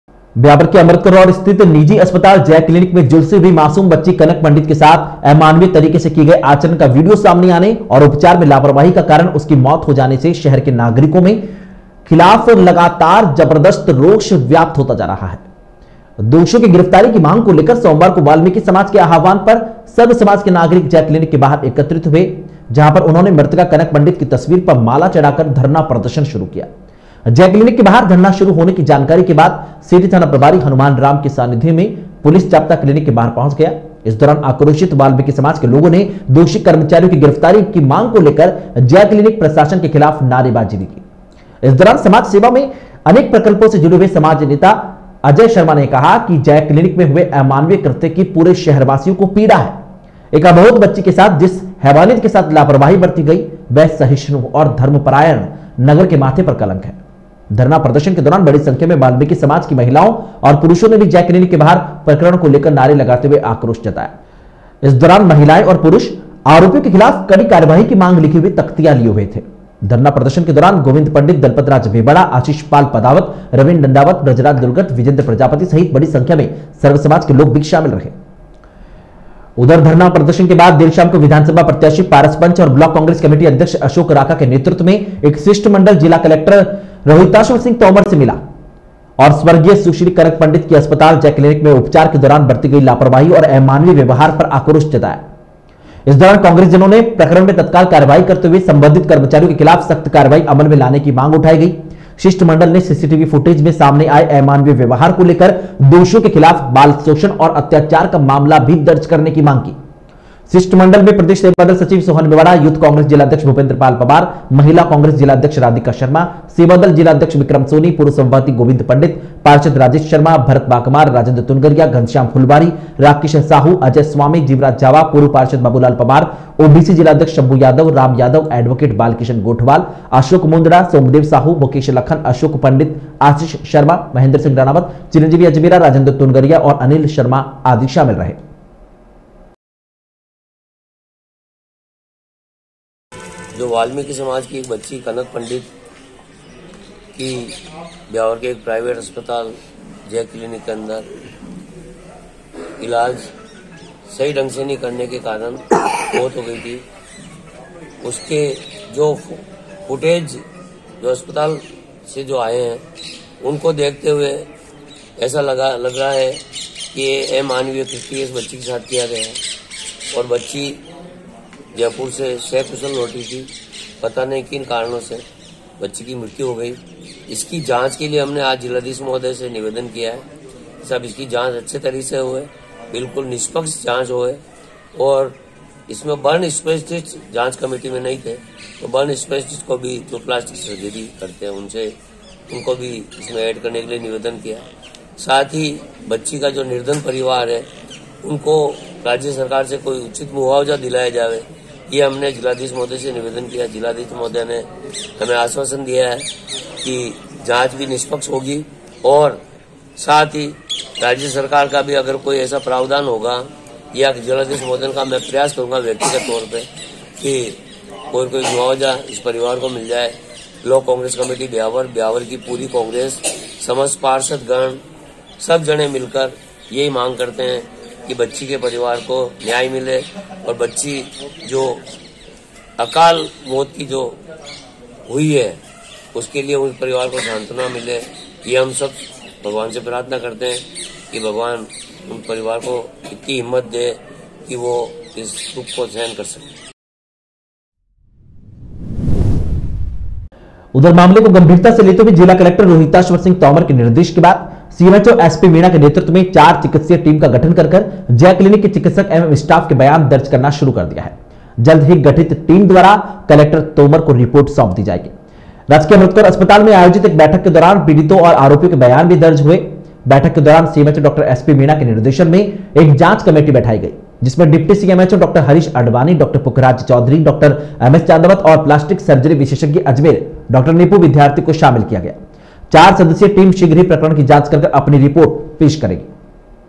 कनक के हत्यारों के खिलाफ शहरवासियों में आक्रोश
कनक के हत्यारों के खिलाफ शहरवासियों में आक्रोश, दोषियों की गिर फतारी की मांग को लेकर क्लीनिक के बाहर दिया धरना